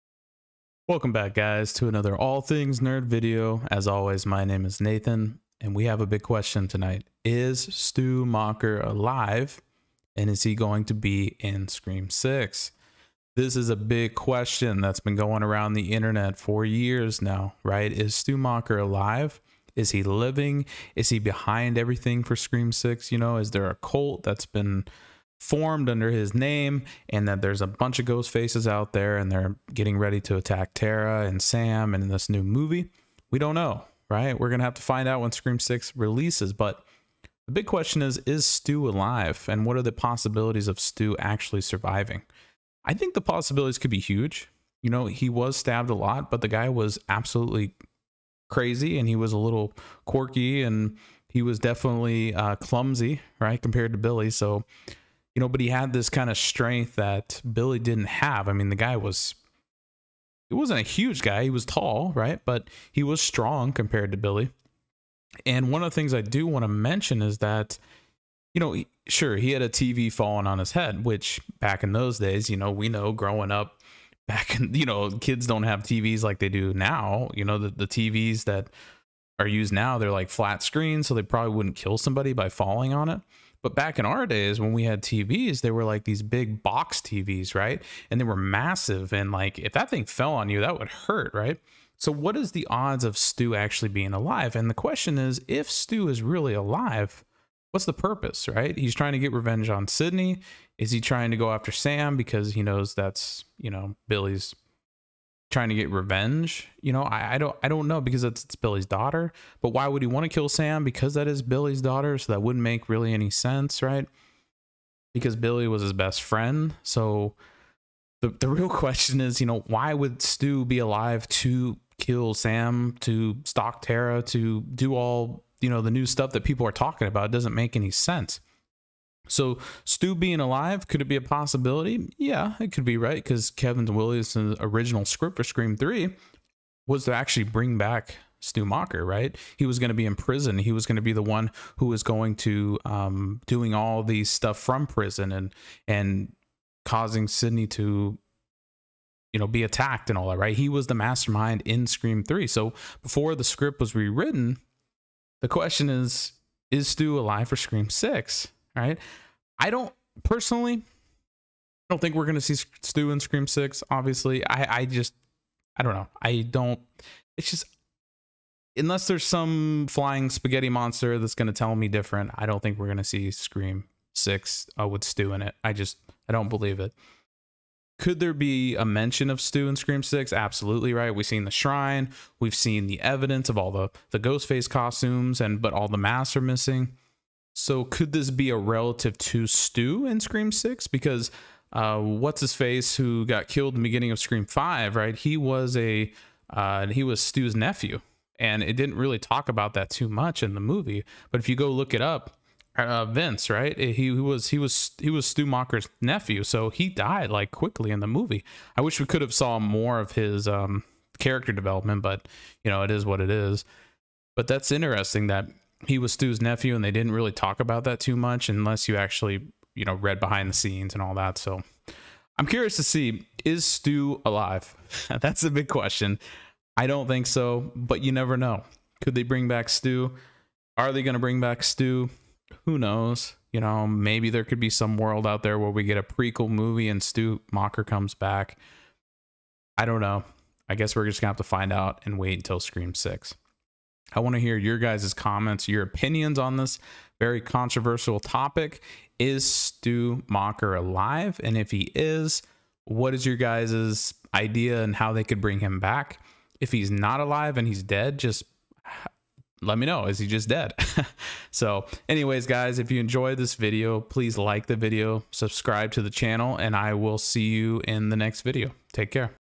Scream 6 | Is Stu Macher ALIVE or DEAD?
In this Scream 6 breakdown, we investigate the potential alive or dead status of Stu Macher. After watching Scream, do you think he's alive or dead?